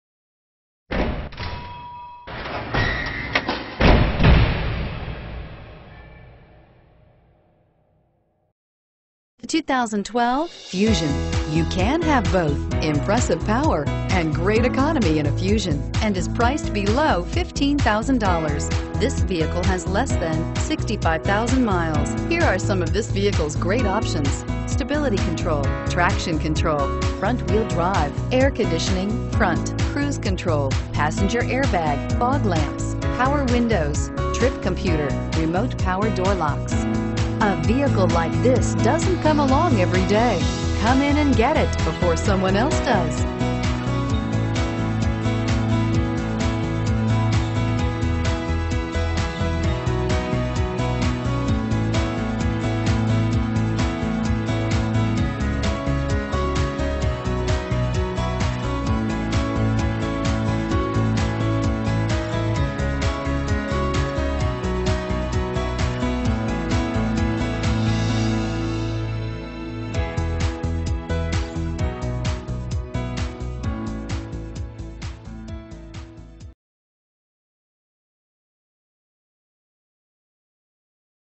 2012 Ford Fusion Cedar Creek, TX CR436689
1586 State Highway 71 West

You'll love this 2012 Ford Fusion. This is a  you'll want to take home. With 64506 miles, it features Automatic transmission and an exterior color of Sterling Gray Metallic. Call and get in touch with Benny Boyd Bastrop directly, and be the first to open the door today!